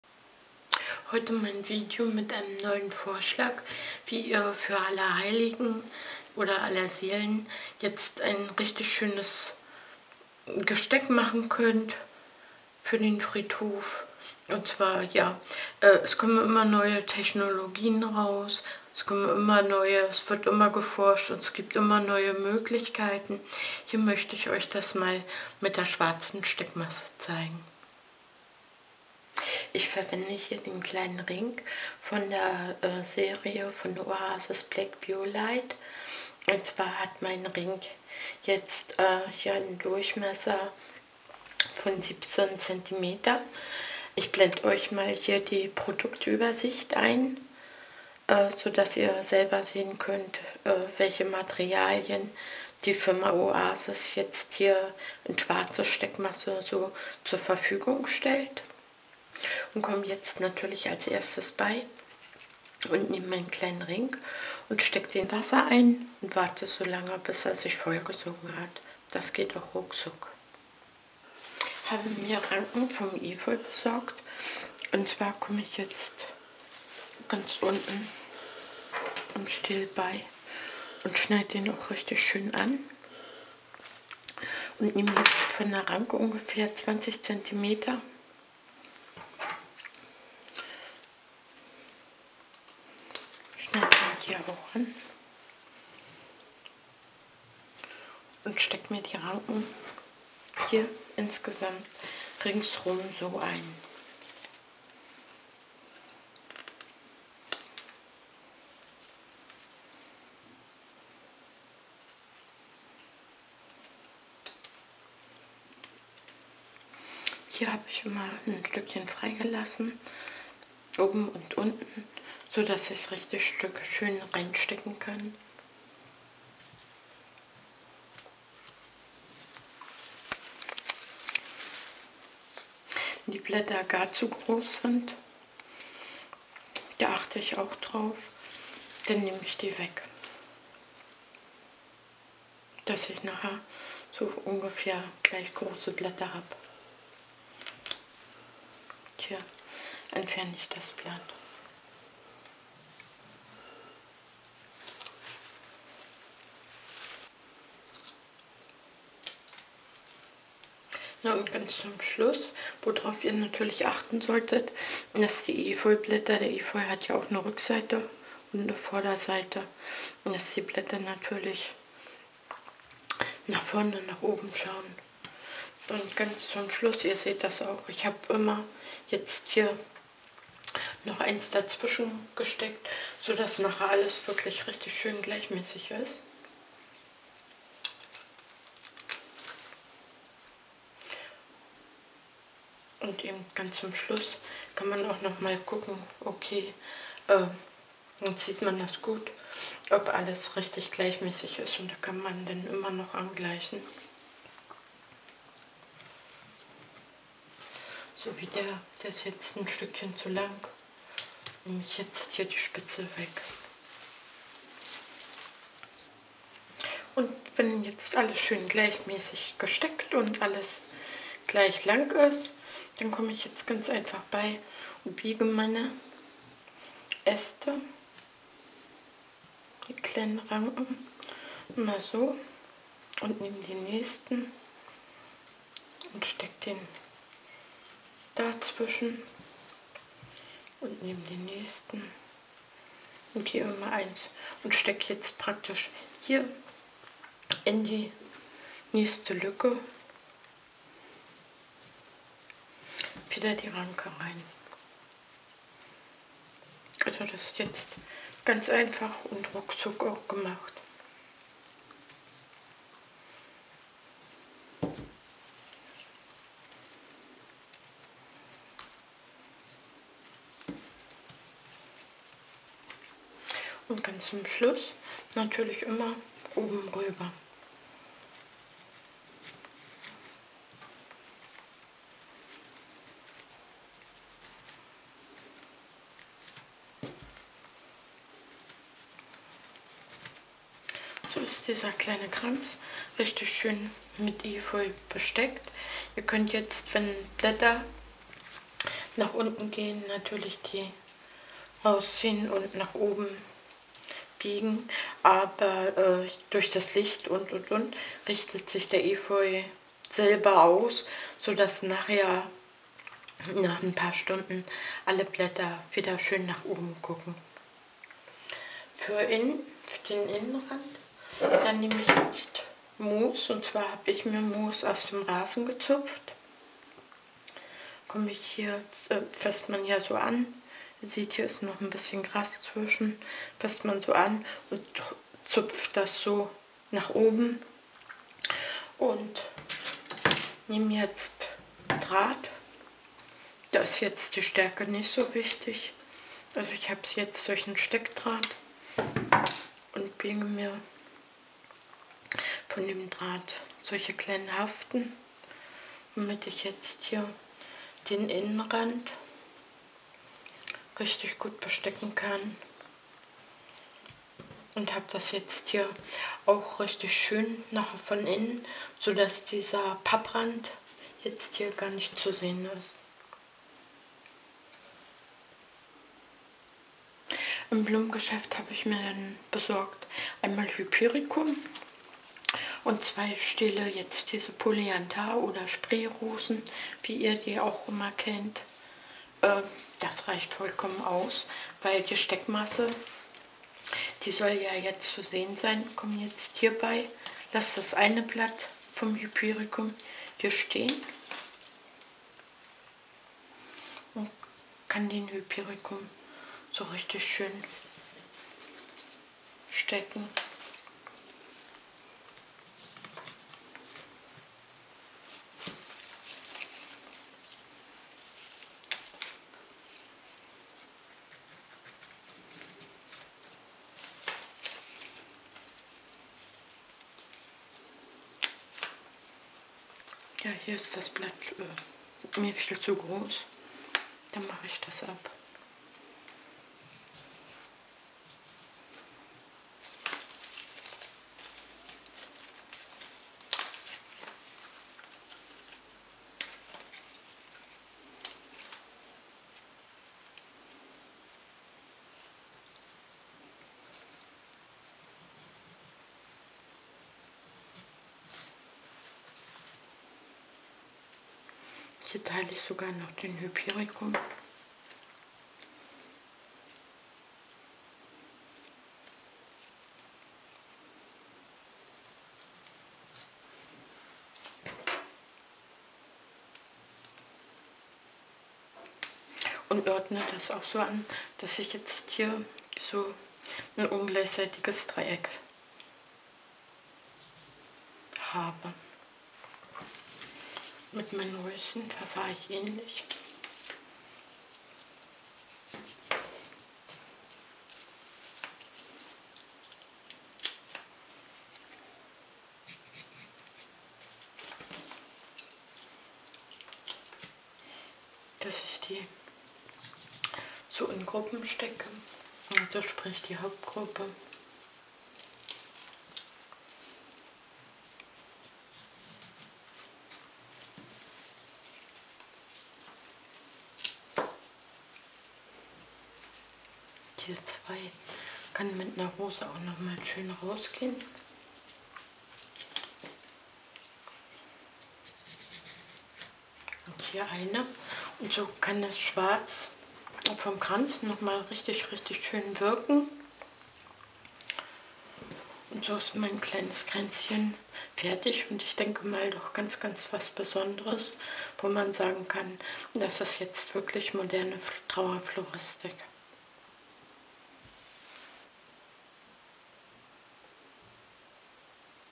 Grabgestecke selber machen, Kranz mit Steckmasse / Oasis Black Biolit ❁ Deko Ideen mit Flora-Shop
In diesem Video zeige ich, wie du einen kleinen Kranz für Allerheiligen selber machen kann.
INFOBOX HIER ÖFFNEN - - - - Klicke auf MEHR ANZEIGEN

Die von mir verwendeten Materialien. *Werbung durch Produktempfehlung

Efeu und Moos aus dem Garten

Habe ich im Blumengeschäft gekauft.
1 x Hypericum
2 x weißfarbene Tros-Rosen, (Polyantharosen) der Sorte "White Bubbles"

Bitte aktiviere die Glocke, um meine Videos zu sehen.